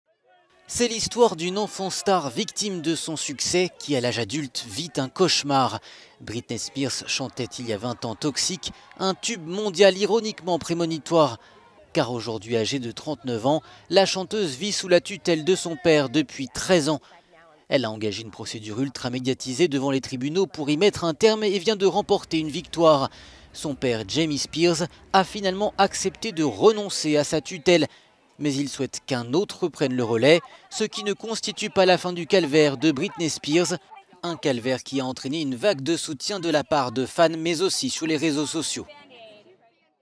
Le père de Britney Spears accepte de renoncer à être son tuteur
Une première victoire pour la chanteuse, qui a lancé une offensive judiciaire ultra-médiatisée pour que son père n'ait plus son mot à dire dans ses affaires personnelles et financières.